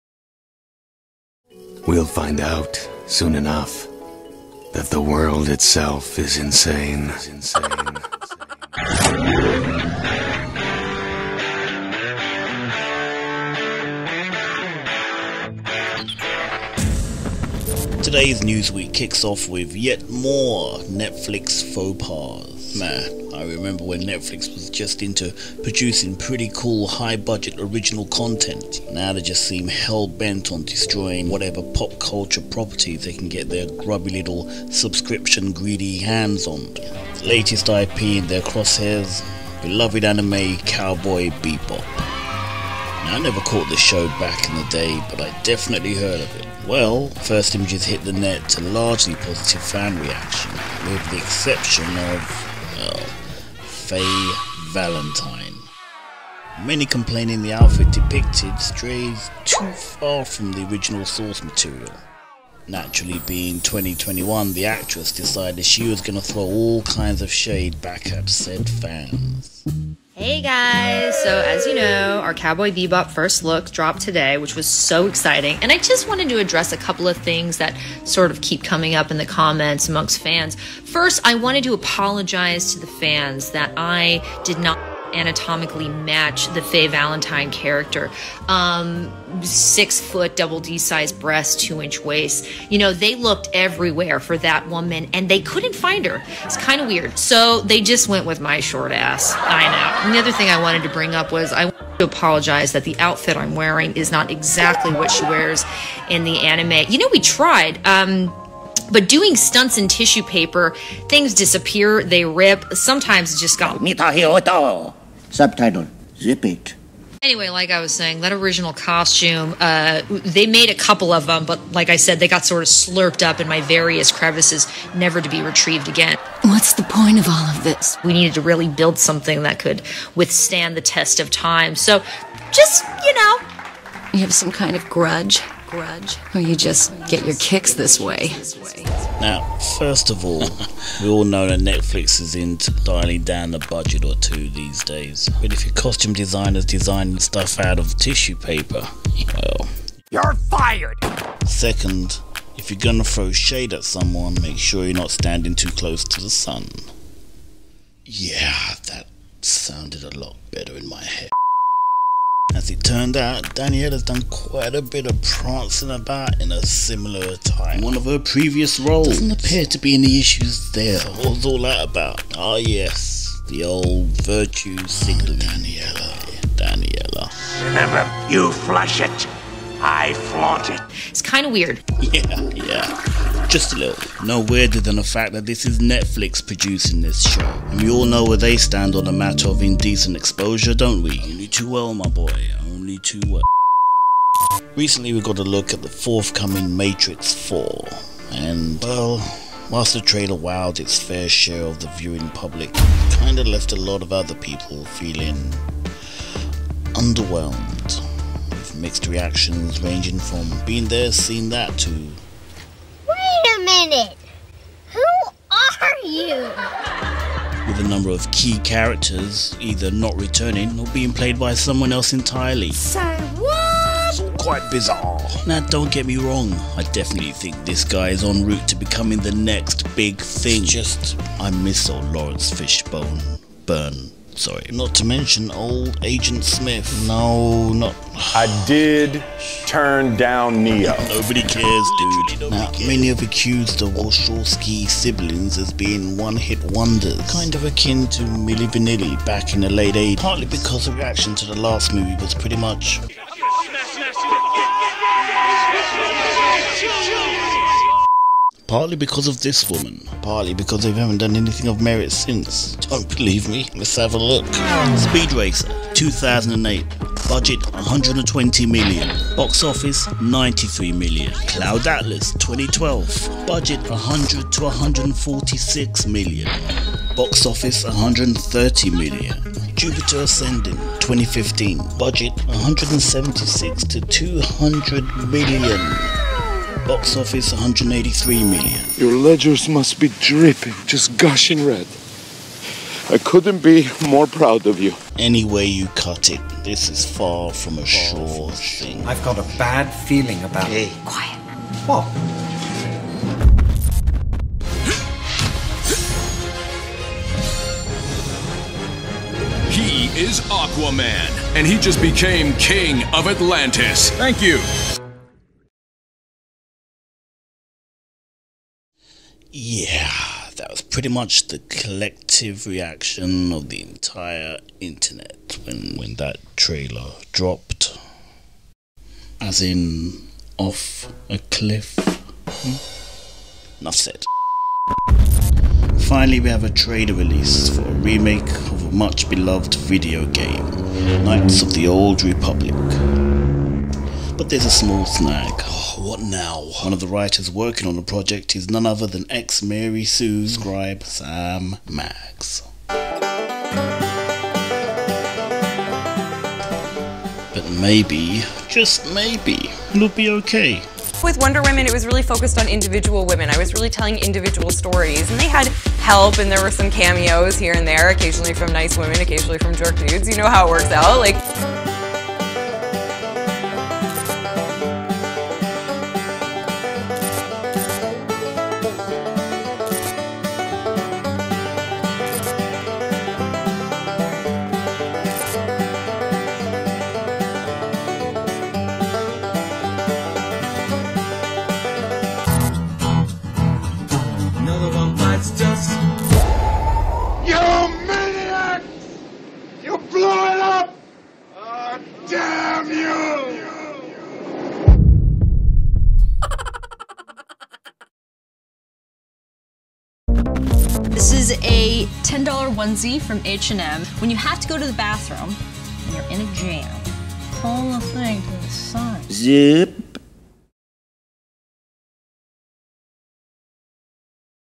Joker's News: Netflix, Matrix & Star Wars woes
Your number one alternative to pop culture-related news, views & reviews!

This week we talk more problems going Netflix's way with the new Cowboy Beebop live action show, Matrix 4 trailer finally hits and beloved Knights of the Old Republic shows off an impressive trailer reveal... before crushing fans of the original beloved game! Thought 2020 sucked? - JW